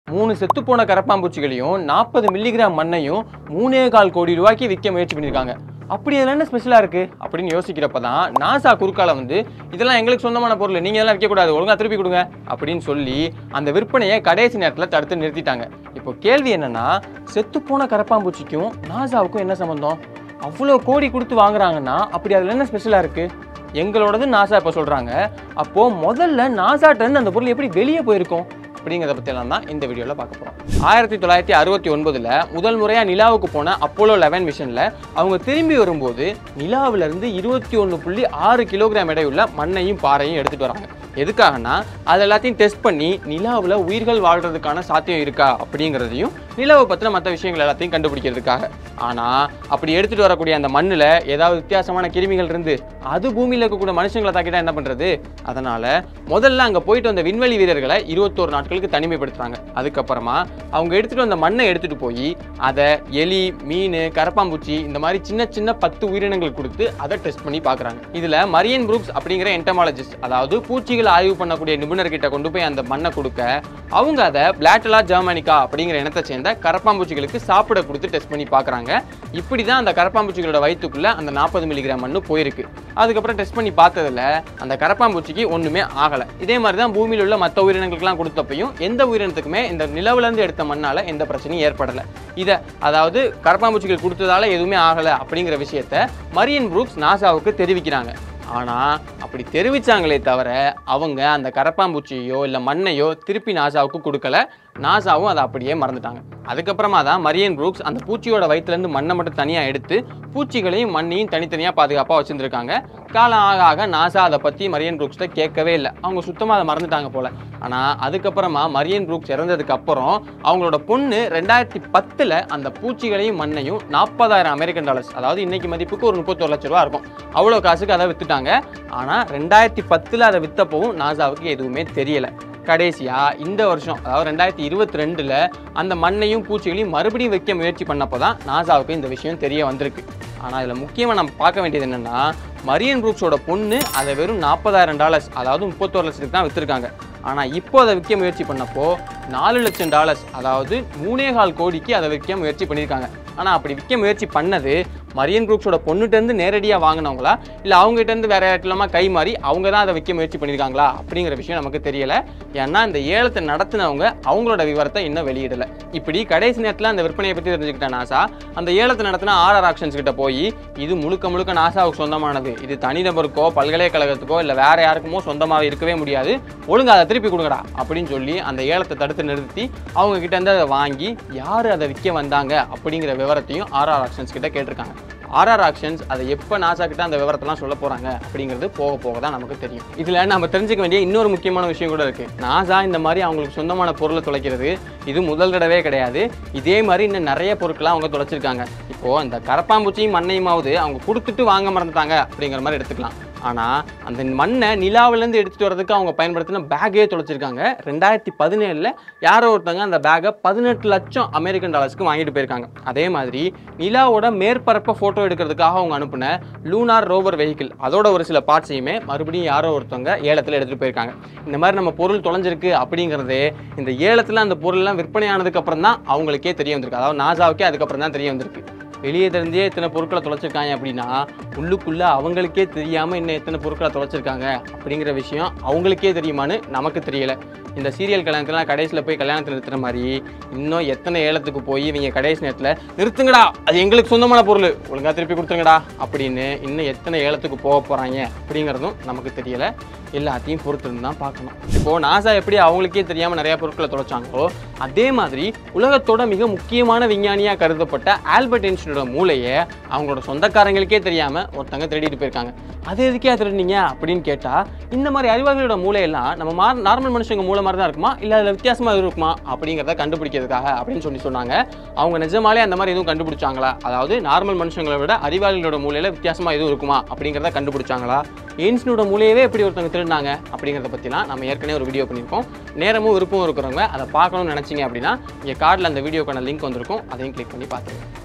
Theft in NASA?? | How NASA lost Lunar soil & research cockroaches? | நாசாவில் திருட்டா?
Recently NASA has found out that its belongings were being auctioned by RR auctions for a huge sum of 400,000 US Dollars. What are those belongings? How did NASA lose them? Why they are valued so highly is explained in detail in this video.

During the Apollo 11 mission of 1969, (moon landing mission) the astronomers brought back around 21.6 kilograms of Lunar soil & rocks to test whether the Moon can sustain life & other important details about the moon. To avoid any germ contamination & spread of diseases due to the soil, the astronomers were quarantined for 21 days after the landing. The soil was sent to a few scientists to test them with a few little organisms like Rat, Fish & Cockroaches. What happened to those animals after feeding them with the Lunar soil? What happened later to that Lunar soil & the animals tested - these are the other things explained in detail in this video